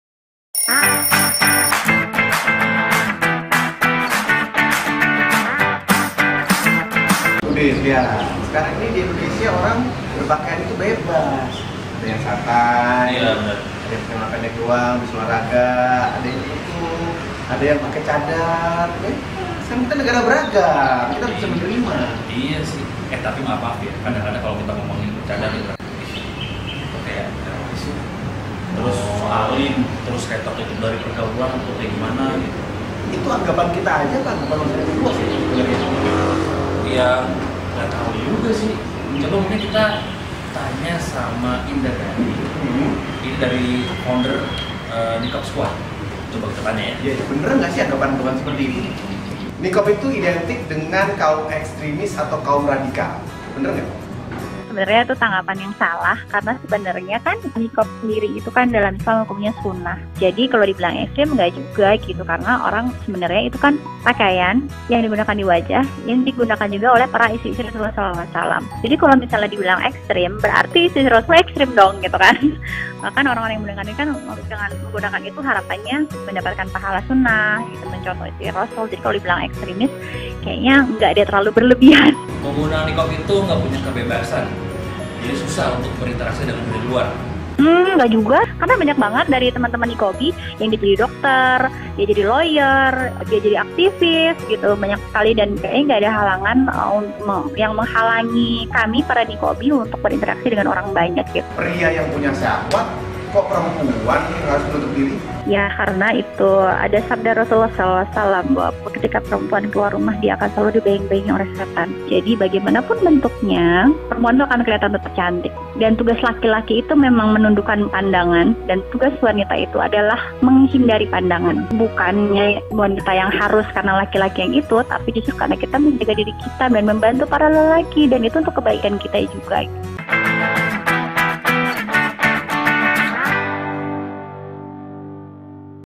Stigma Niqab
Apa yang terbesit di benak Anda jika melihat perempuan bercadar?

Sejumlah stigma menempel pada orang yang memilih untuk menutupi sebagian wajahnya dengan cadar atau niqab. Disebut sok-alim, tidak mudah bergaul hingga dicap bagian dari jaringan teroris.

Founder Niqab Squad, Indadari menjawab anggapan-anggapan miring yang menempel pada cadar. Apa katanya?

*Niqab Squad adalah komunitas wanita bercadar Indonesia. Anggotanya disebut niqabis karena semuanya memakai niqab.